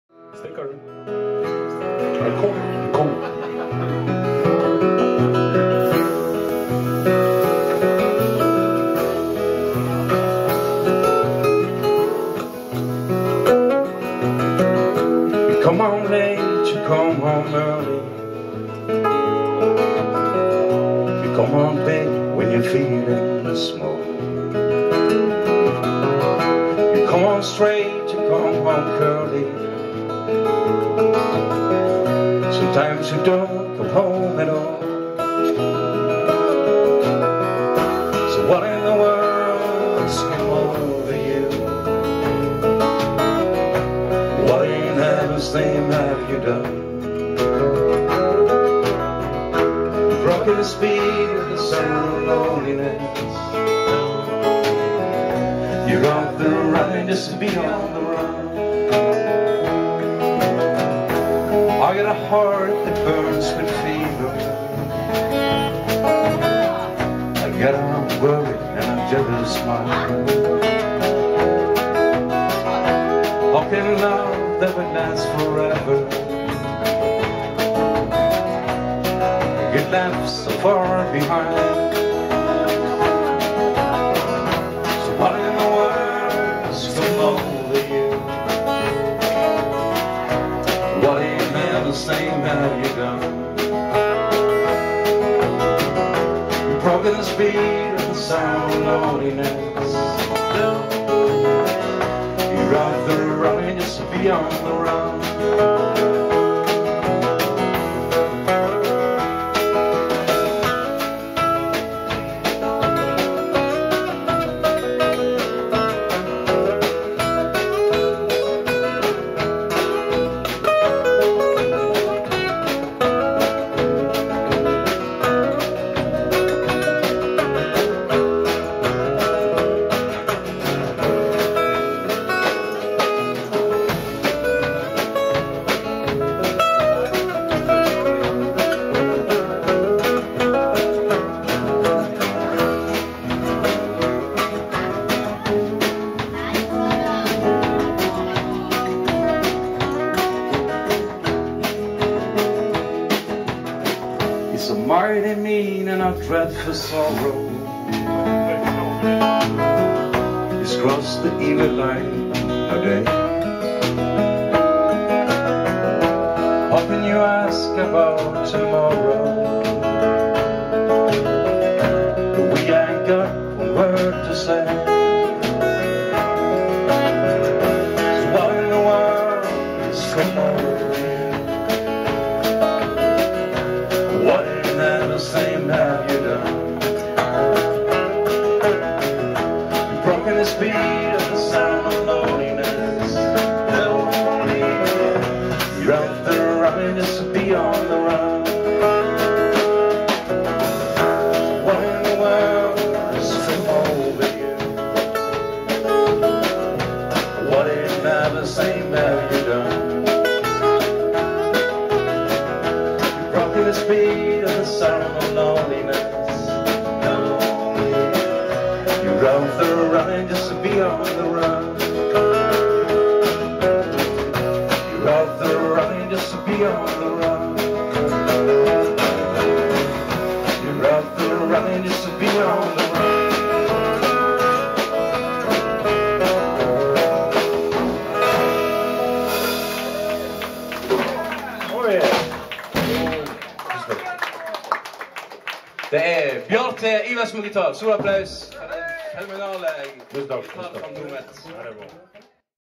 BRØDRENE IVERSEN | Dyvekes Viseclub
Dyvekes: 17.mars -11 -  DAGFNN IVERSEN vocal - BJARNE AASMUL gitar
GEIR ROGNØ bass